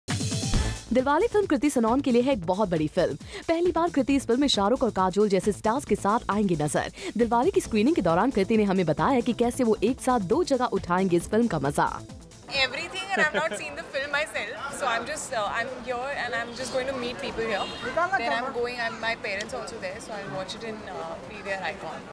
Kriti Sanon Feels Excited About 'Dilwale' | Bollywood News
Dilwale is a big film for Kriti Sanon & she is all excited for it.